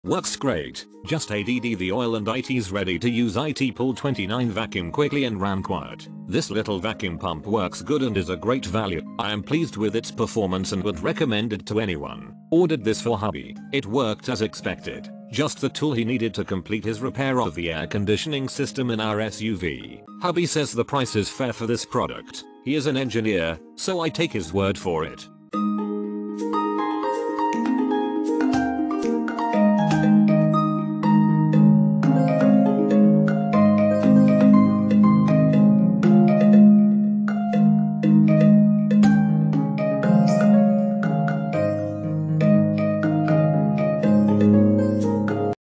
ARKSEN Rotary Vane Deep Electric Vacuum Pump HVAC Single Stage, 110v, 1//3HP, 4 CFM review
Main Features:
by entering your model number.. Quickly and effectively removes moisture and air arom air conditioning system. Lightweight cast aluminum construction for maximum durability. Direct drive motor allowing easy starting and is maintenance free. Oil viewing windows allows you to monitor oil quality and levels. Built-in high volume cooling fan for extended use